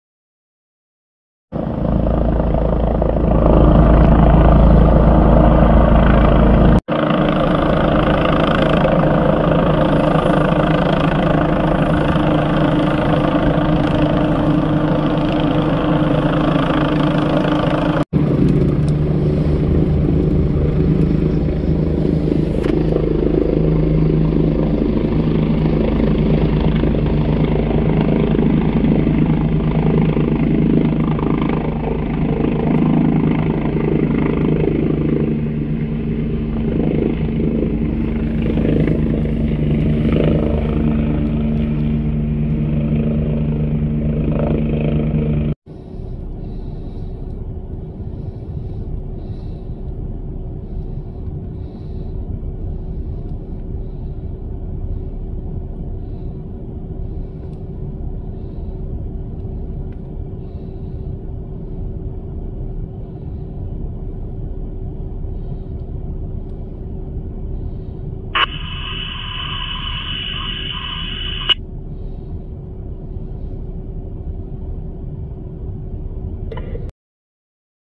LifeNet Medevac Arriving at MidHudson Regional Hospital
LifeNet arrived to transport a five-year-old boy to WMC but the child passed away.